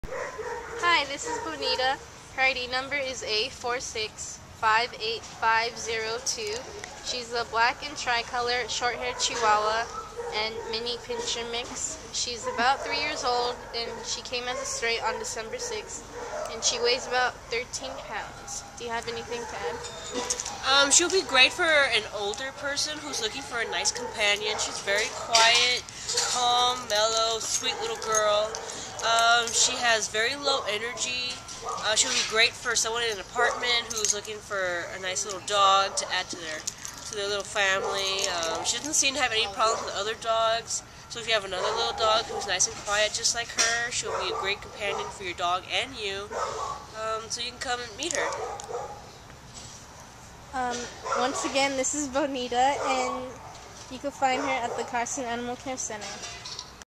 Bonita A4658502
A4658502 Bonita is a serene three year old tricolor female shorthaired Chihuahua / Miniature Pinscher mix who was found in Los Angeles on December 5th and brought to the Carson Animal Care Center.  Weighing thirteen pounds, she is excellent on leash, gets along with other dogs her size and likes being held.  She is a well-balanced girl with quiet, mellow energy who will make a great lap dog for a senior in an apartment looking for a cuddle-bug companion.  Bonita is the perfect indoor pet for anyone in any living situation.